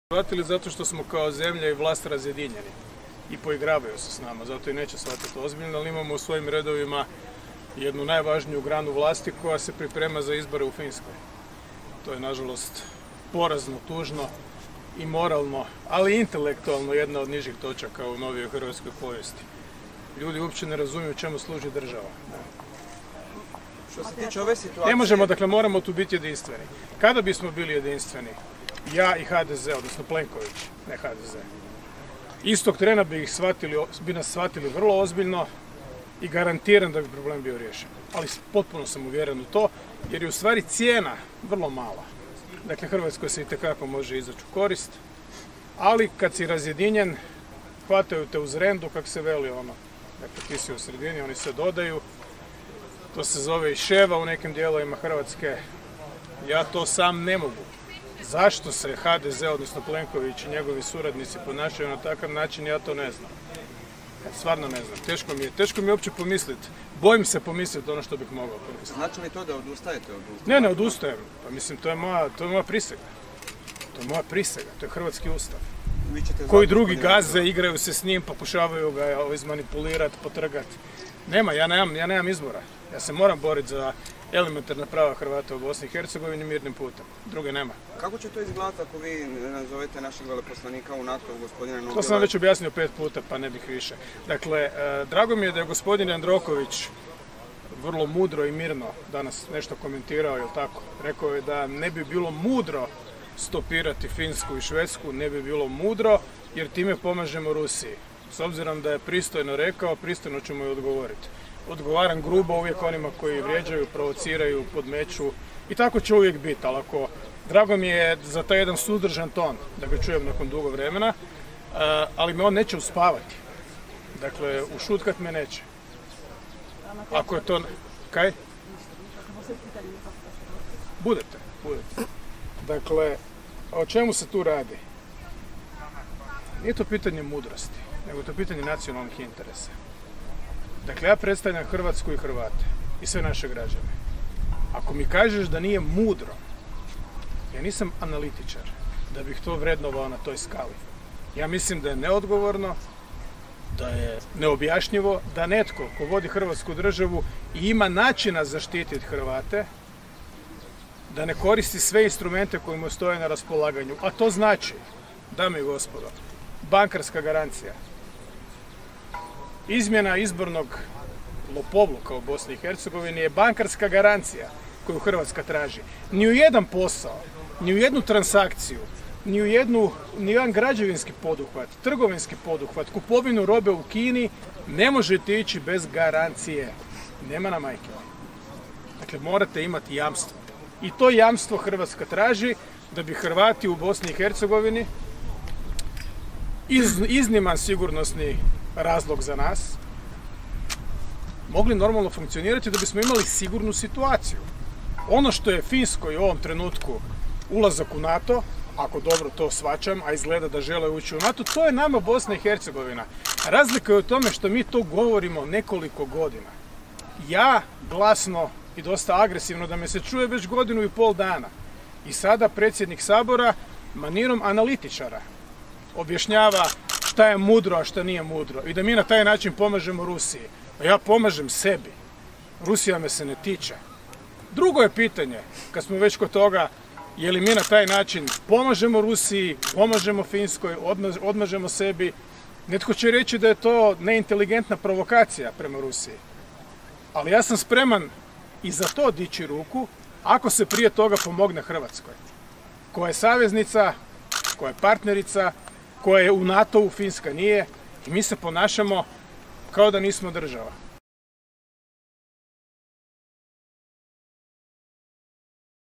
Milanović: Ono što je Finskoj ulazak u NATO, nama je Bosna i Hercegovina
Predsjednik Republike i vrhovni zapovjednik Oružanih snaga Republike Hrvatske sudjelovao na obilježavanju 31. obljetnice osnutka 2. gardijske brigade „Gromovi“ i dodjeli beretki pripadnicima 2. mehanizirane bojne "Gromovi" u vojarni "Pukovnik Predrag Matanović". Nakon dodjele beretki  predsjednik Zoran Milanović dao izjavu za medije.